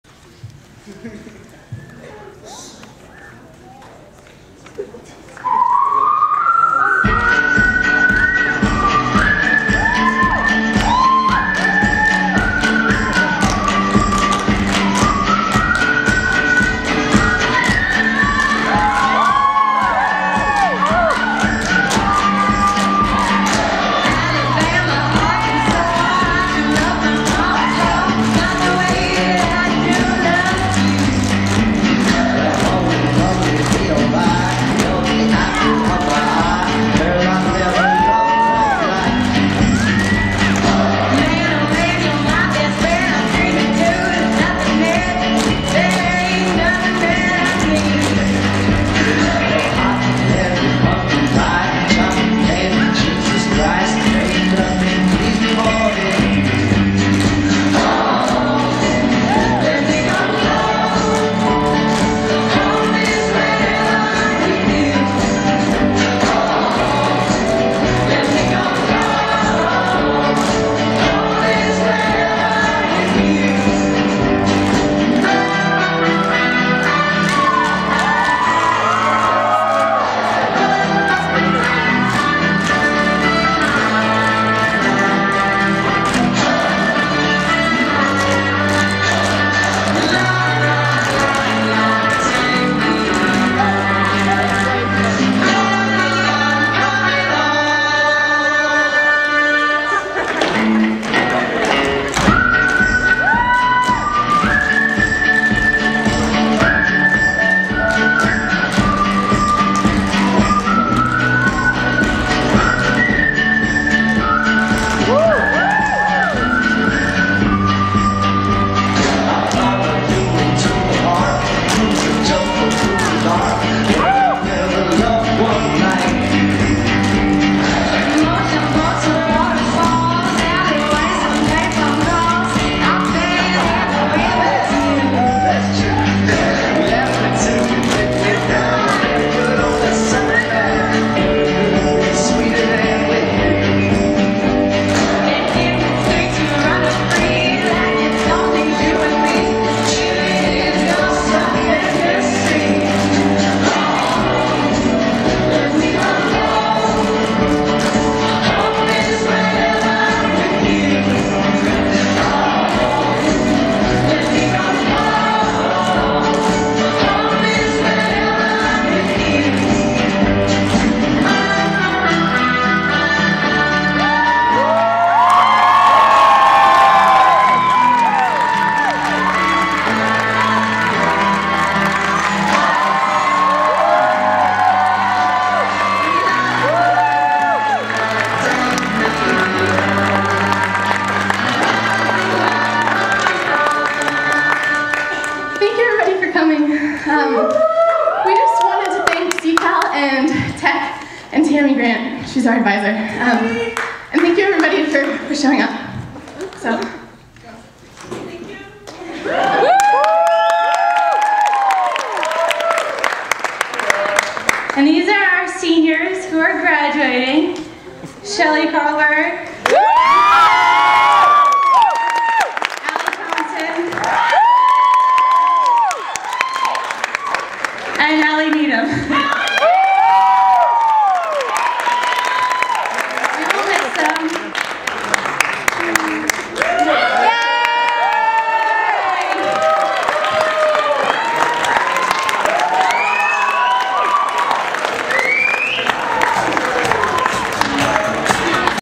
Home
Choreography by Julianna Mauriello
Performed by: Shelley Carlberg, Hanna Howell, Thomas Huang, Carllee James, Alana Jenkins, Nora Lamm, Julianna Mauriello, Allie Needham, Rachel Nuñez, Hannah Stork, Laura Strom, Ali Thompson